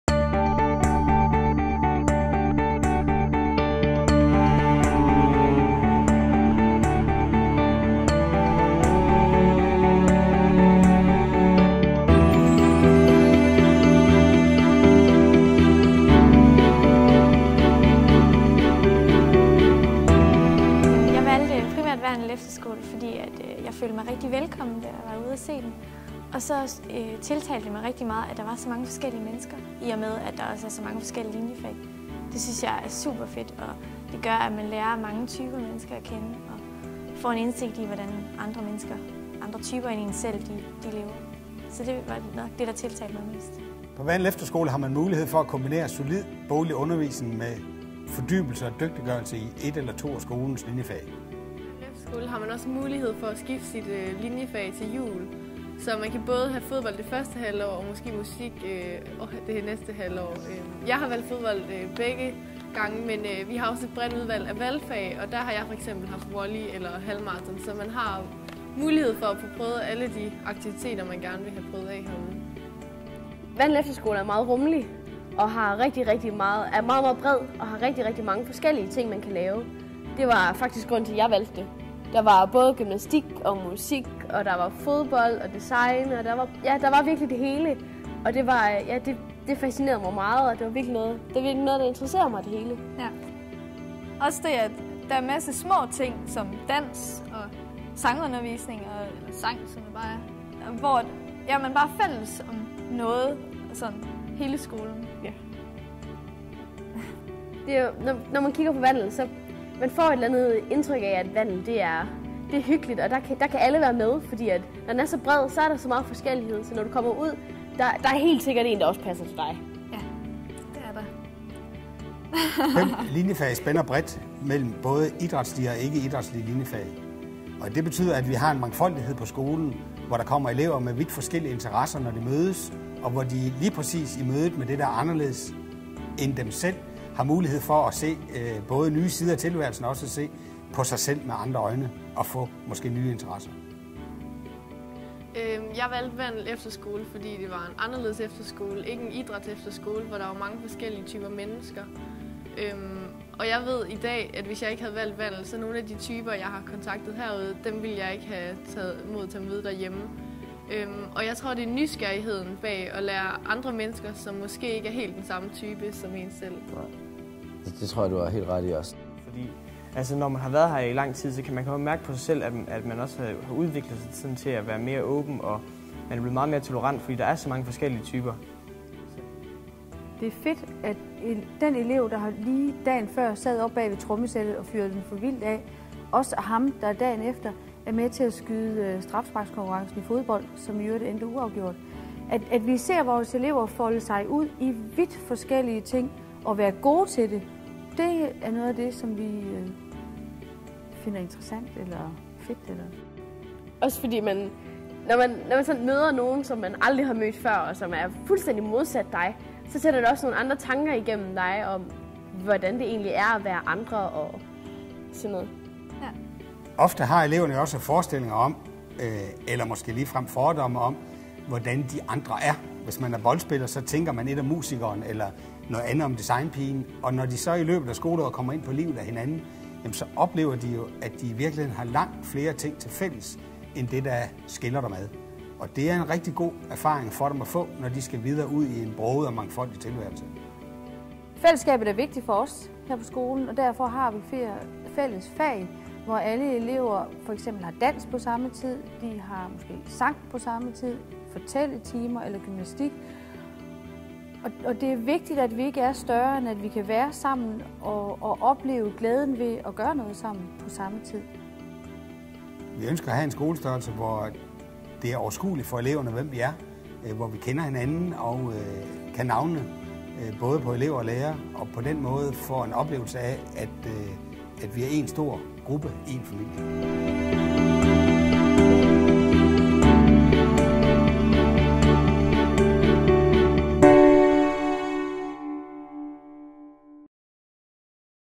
Vandel Efterskole Skolefilm
Mangfoldighed, nærvær og solid boglig undervisning er det vi er kendt for på Vandel Efterskole